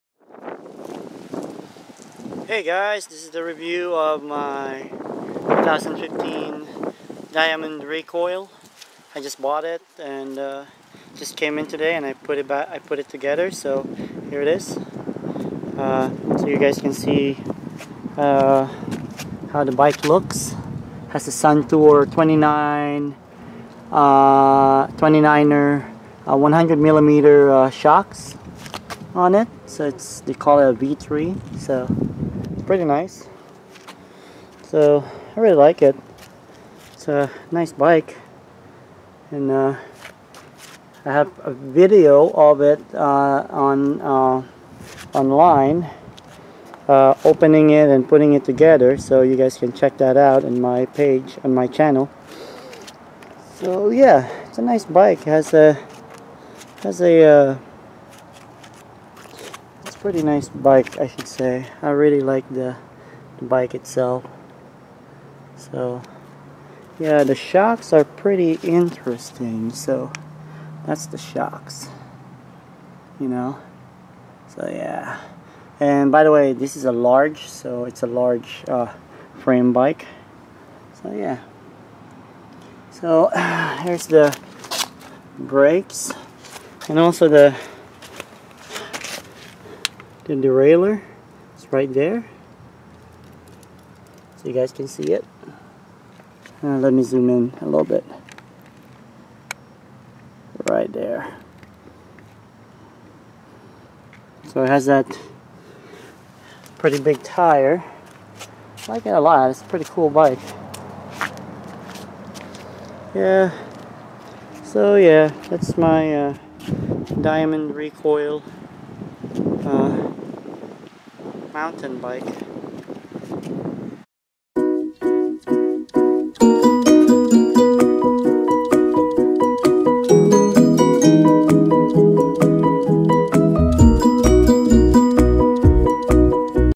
Diamondback Recoil 29er HOW IT LOOKS LIKE
This is my new Diamondback Recoil 29er 2015.

100mm sun tour sr 29er forks
Large frame size 20"
Oil shocks springs